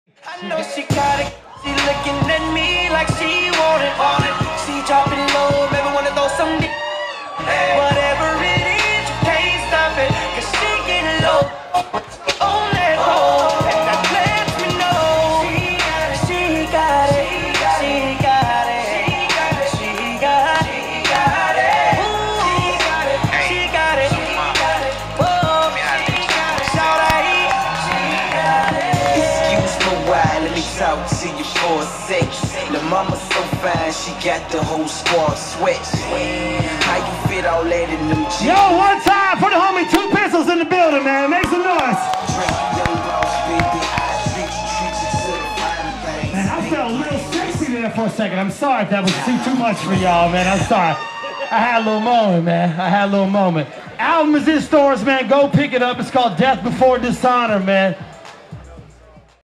DJ Mingle Mixx King Gators 4th of July Spectacular
A Drop and Intro for 2 Pistols during the 6 July Bikini Contest at Gators Cafe (kinggatorvision on youtube)